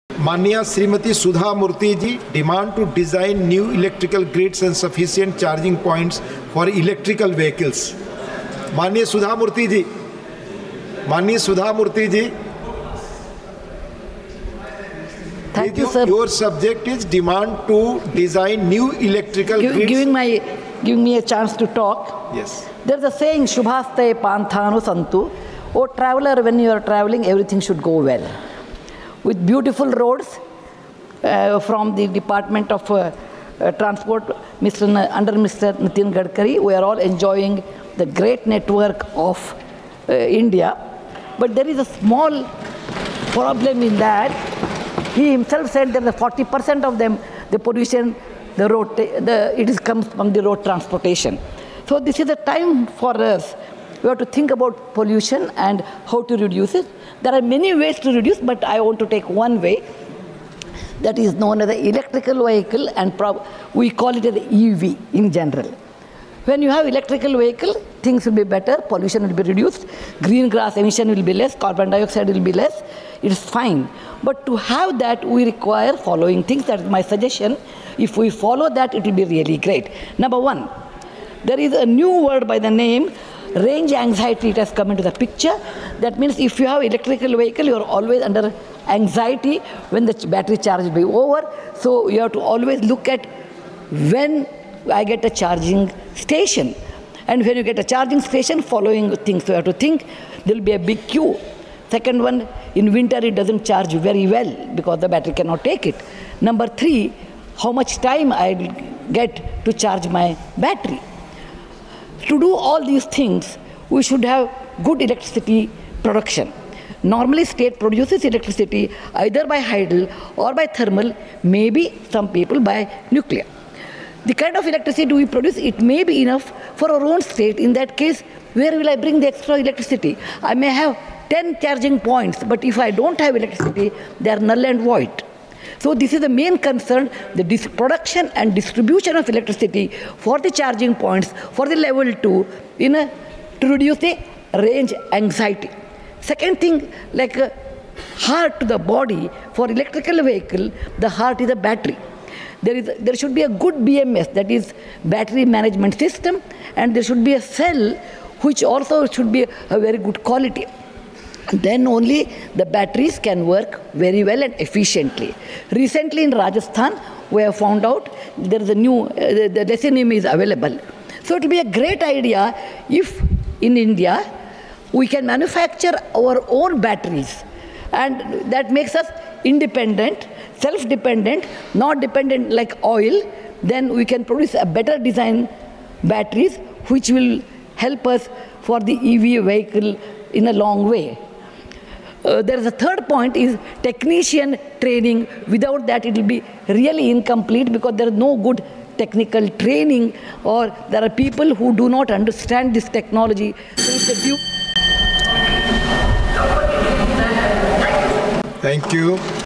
Monsoon Session | Rajya Sabha MP Sudha Murthy का Nitin Gadkari से 'विशेष' अनुरोध !
मनोनीत सांसद सुधा मूर्ति ने राज्यसभा में बोलते हुए इलेक्ट्रिक वाहनों (ईवी) की चार्जिंग प्रणाली को लेकर केंद्रीय मंत्री नितिन गडकरी से विशेष अनुरोध किया।